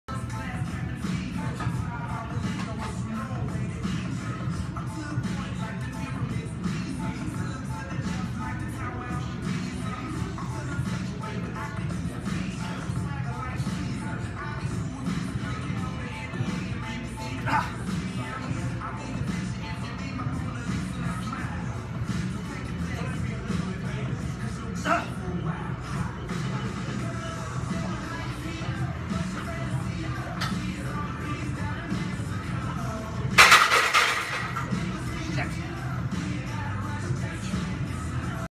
Front squat 4@300 and a missed 5th
Overestimated my strength level. Bar slipping down right shoulder. Followed up with 2x5@290.